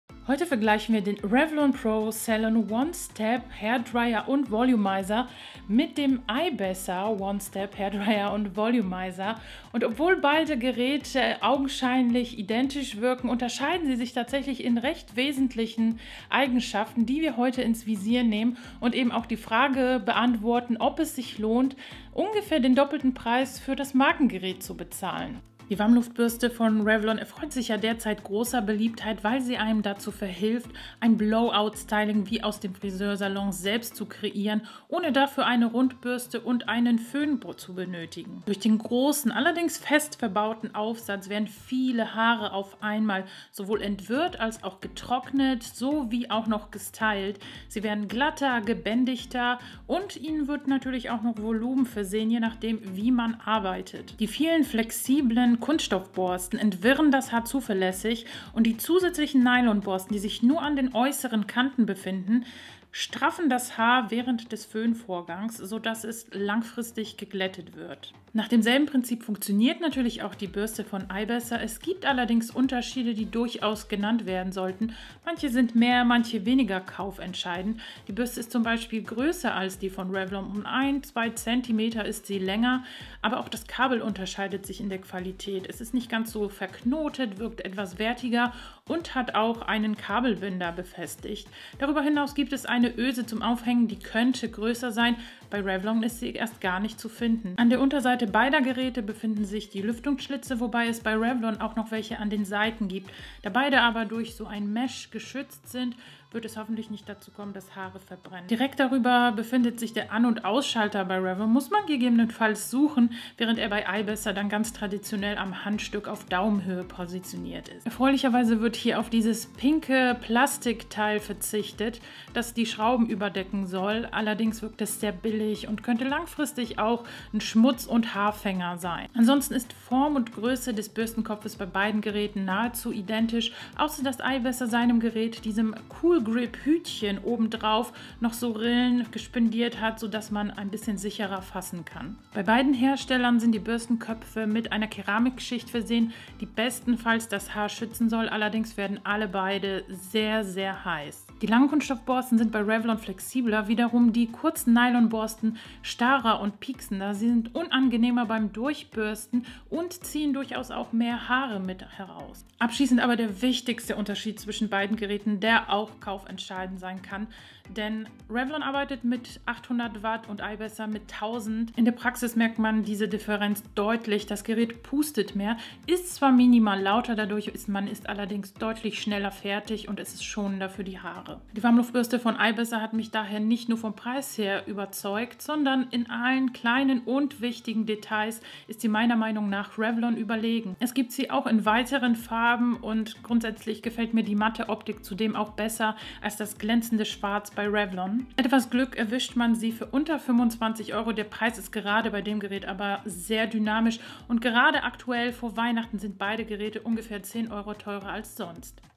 REVLON One Step Hair Dryer & Styler vs. AIBESSER Warmluftbürste im Vergleich (Adkwse)
REVLON vs. AIBESSER Warmluftbürste im Vergleich - optisch sehr ähnlich, im Detail gibt es jedoch Unterschiede, die auf eine klare Kaufempfehlung hinauslaufen.

AIBESSER
bei Amazon ➜

ADKWSE (augenscheinlich baugleich zu Aibesser)
bei Amazon ➜

Am besten funktionieren solche Jumbo-Warumluftbürsten bei dichterem, mittellangem Haar. Man sollte sie bestenfalls vorher mit Hitzeschutz behandeln, gut durchbürsten und auch vortrocknen, sodass sie der direkten Hitze nur für das Styling ausgesetzt werden. Mit einem Gewicht von rund 450g sind sie auch schwer  (typische Warmluftbürsten wiegen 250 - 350g) und es kann müßig sein sie lange nach oben zu halten. Auch die 1-3dB mehr Lautstärke machen sich bemerkbar.

Beide Geräte wurden mir zu redaktionellen Zwecken ausgeliehen. Ich werde mir keines davon nachkaufen, weil sie für mein schütteres Haar keine Vorteile mitbringen. Derzeit sind keine weiteren Videos dieser Art geplant.

Beim REVLON Pro Salon One-Step handelt es sich übrigens um ein Lizenzprodukt. Haargeräte der Firma werden von "Helen of Troy" vertrieben. Es ist also kein Produkt, das direkt von der bekannten Firma entwickelt wurde. Ob AIBESSER und andere "No-Name-Geräte" Kopien sind oder man das REVLON-Gerät nur branden hat lassen, ist nicht klar. Man sollte keine innovative Technik wie beispielsweise beim DYSON Air Wrap erwarten.

SCHAUT AUF MEINEM HAUPT-CHANNEL @magimania VORBEI!